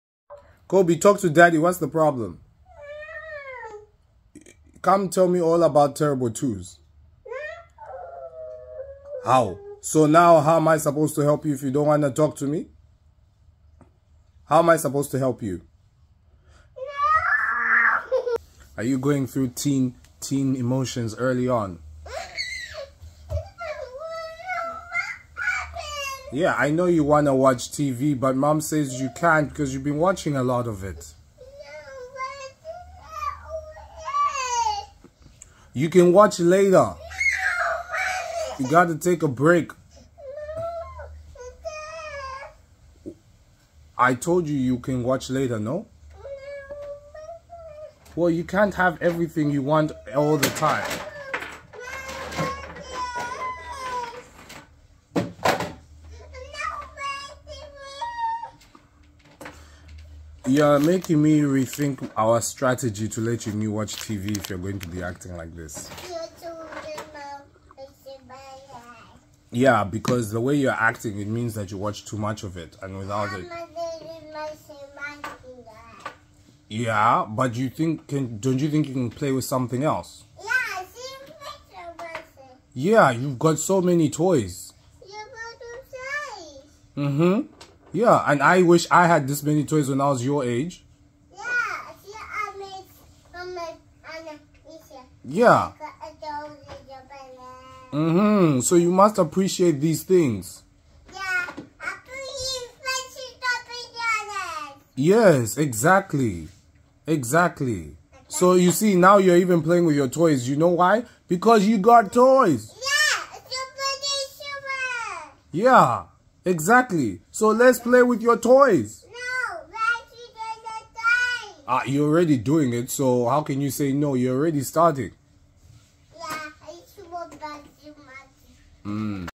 When Parents Say No to a 2 Year Old
Mum told my son Kobe no more TV. He came to complain to Dad, and Dad had to reaffirm what mum said. He throws a tantrum. Dad decides to talk it through with him.